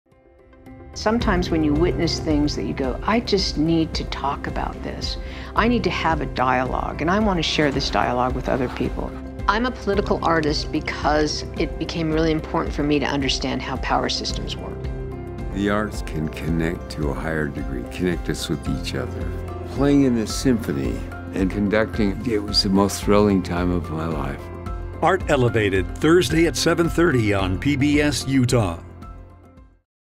V. Kim Martinez & Jack Ashton | Art Elevated: The Governor’s Mansion Artist Awards
Art is both something that exists in the present, as well as a gift to be given to the future. Muralist V. Kim Martinez, inspired by the Mexican muralists of the 20th Century, now passes her craft along as a professor &community activist. Jack Ashton, longtime violinist for theUtah Symphony, is honored for his work in education, introducing students to the joy of creating music. Watch on PBS Utah, beginning July 14th at 7:30 p.m.!